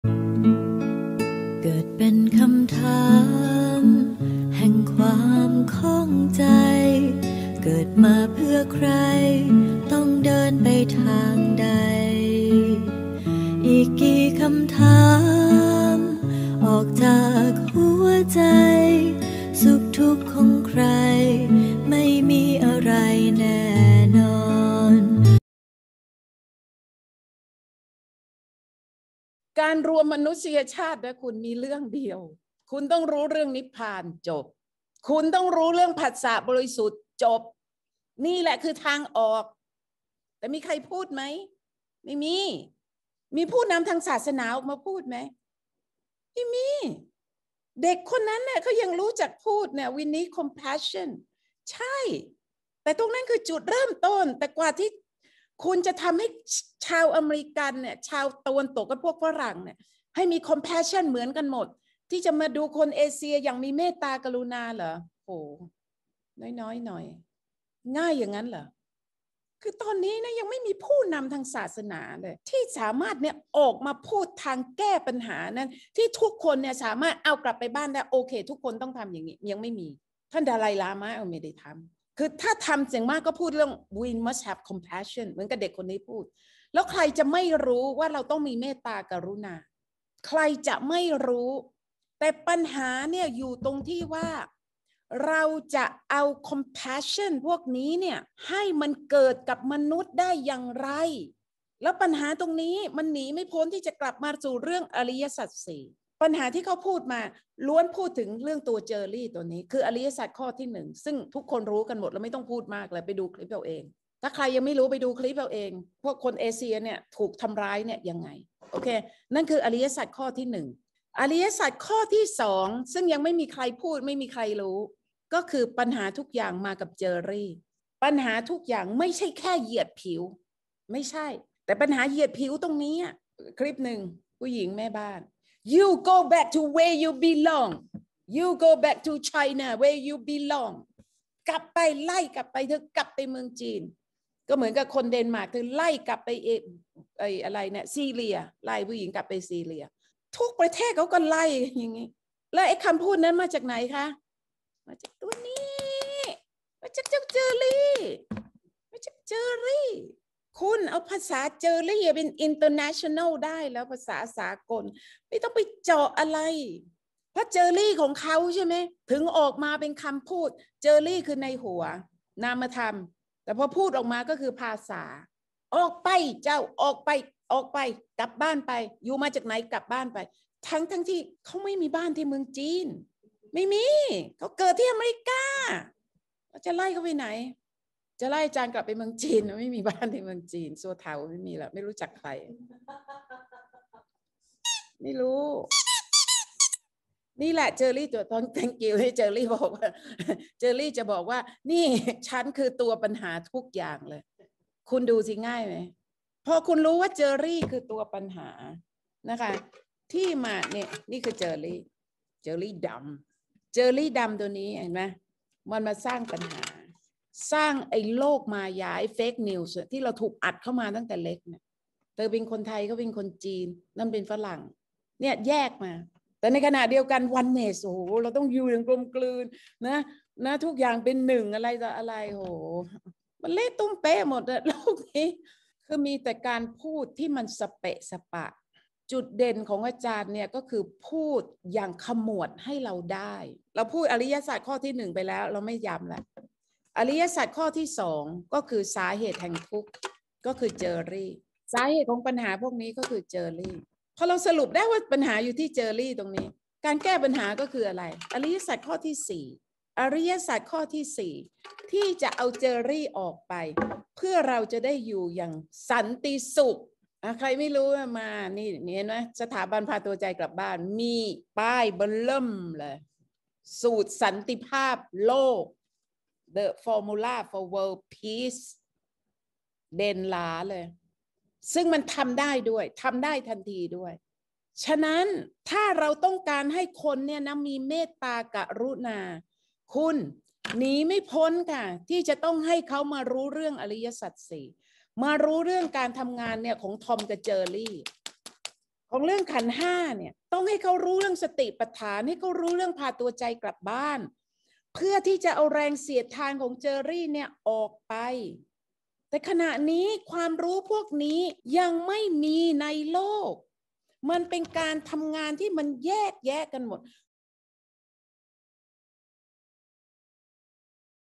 ไวรัสเกลียดคนเอเซีย (ตอน 3) เอาสูตรสันติภาพโลกของศุภวรรณไปใช้ดีไหม
31/3/2564 ณ สถาบันพาตัวใจกลับบ้าน
กระแส “เกลียดคนเอเซีย”  เป็นไวรัสที่กำลังระบาดหนักในอเมริกา
แก้ปัญหาการเหยียดผิว เกลียดคนเอเซีย   จะทำให้คนมีเมตตา กรุณา
ต่อเพื่อนมนุษย์ได้อย่างไร